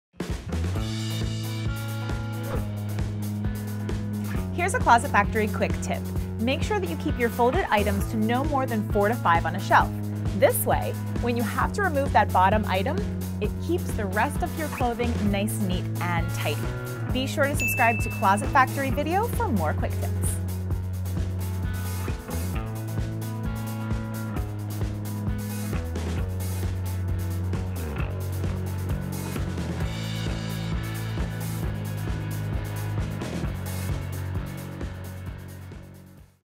Closet Organization: The Right Way to Stack Your Shelves
Closet Organization. On this week's episode of Closet Factory Tips, Christy Biberich teaches you the right way to stack the shelves inside your closet.